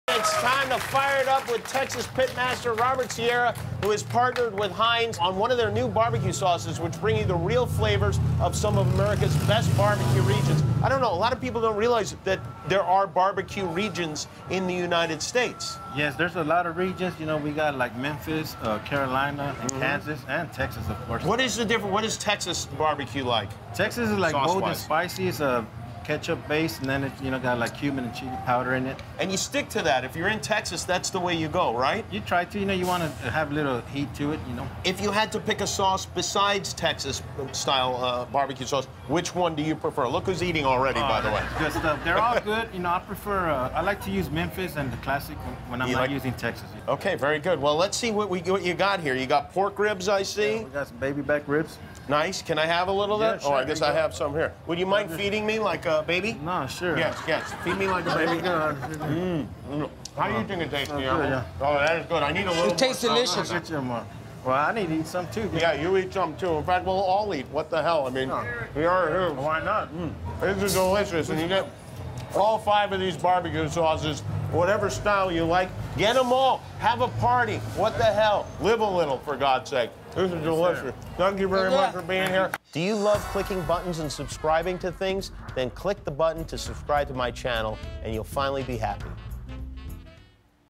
Heinz BBQ Cooking Demonstration with Robert Sierra, Jimmy Kimmel and Guillermo
Texas pitmaster Robert Sierra teaches Jimmy and Guillermo about different BBQ regions in the US.